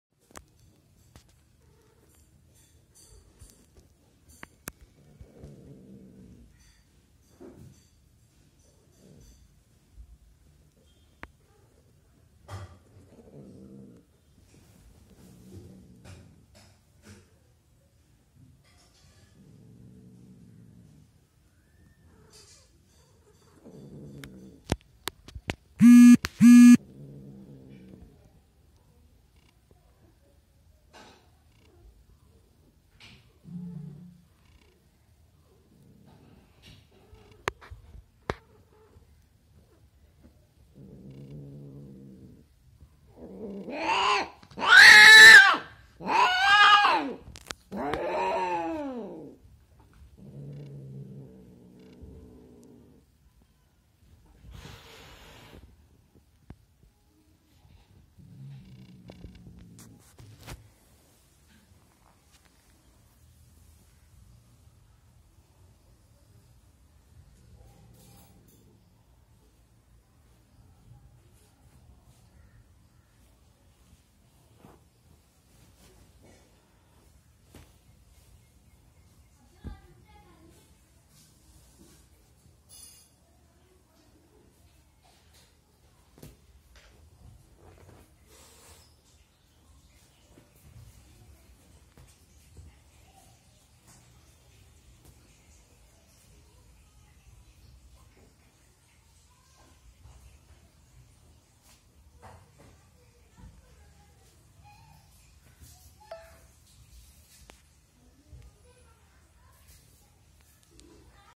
Cat Mating
My cat was having her first heat, so I arranged a Siberian male for him.